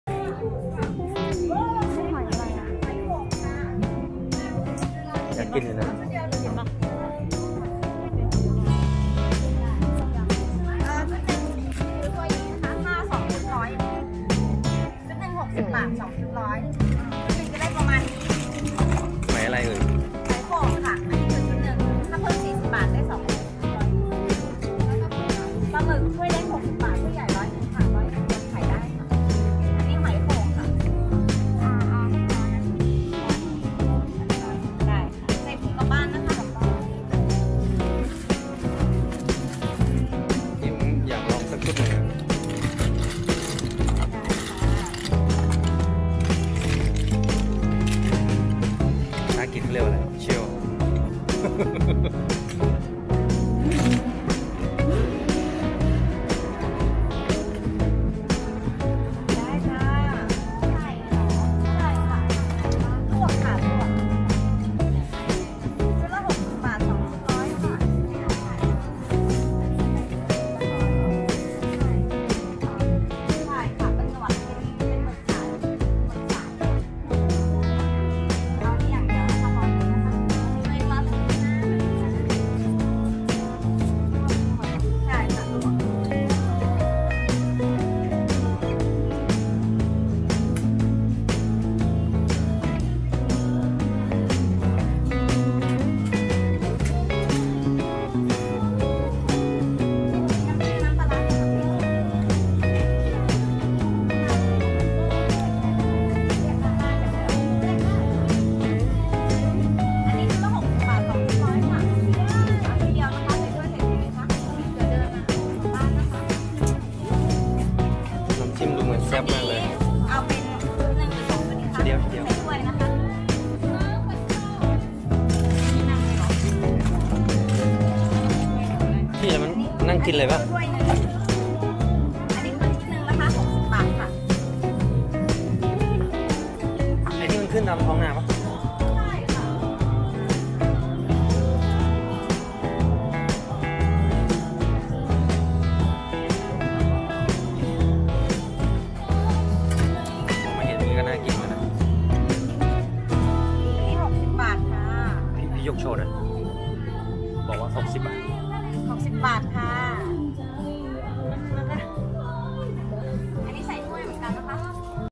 thai street food | Apple Snail With Spicy Sauce | หอยโข่งลวกจิ้ม 60บาท ตลาดหัวมุม
thai street food | Apple Snail With Spicy Sauce